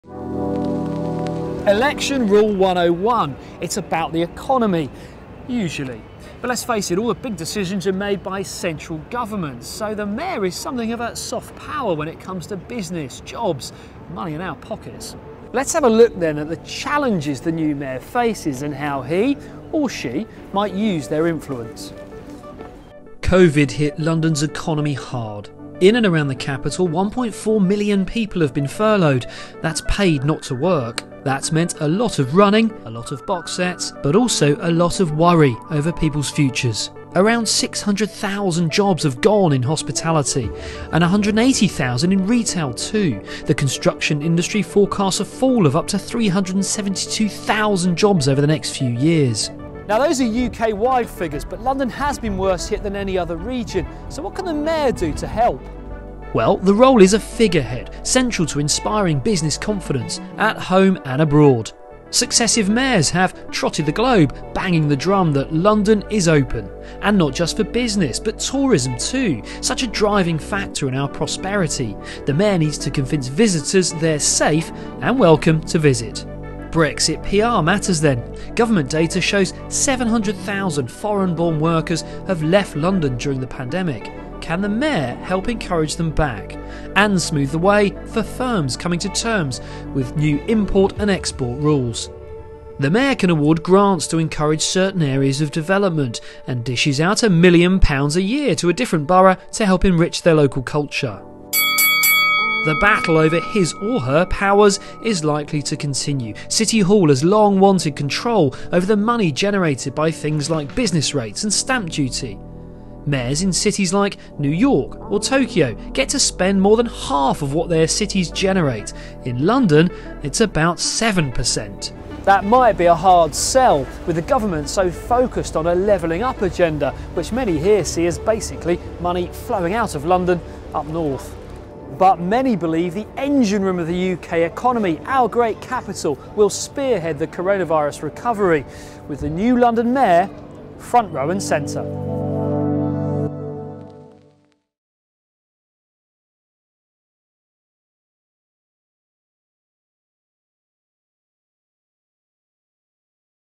Election 2021: The mayor and London's economy
The next London mayor will have a big job revitalising London's post-Covid economy. But just how tough is the job ahead, and how much power does the role possess?